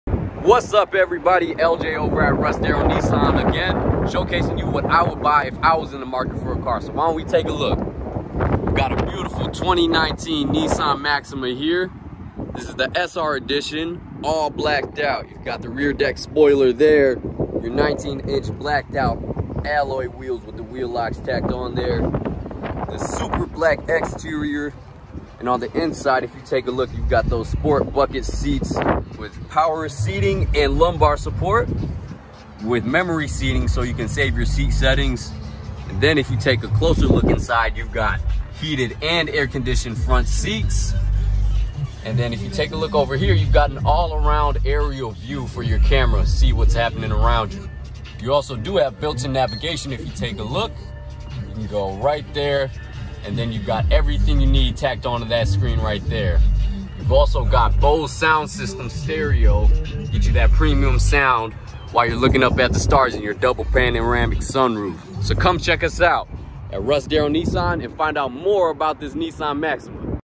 What Car Would You Buy with Russ Darrow Nissan
Get to know your darrowdifferencemaker a little bit better by finding out what vehicle they would get if given the choice! Personally we think they made a great choice.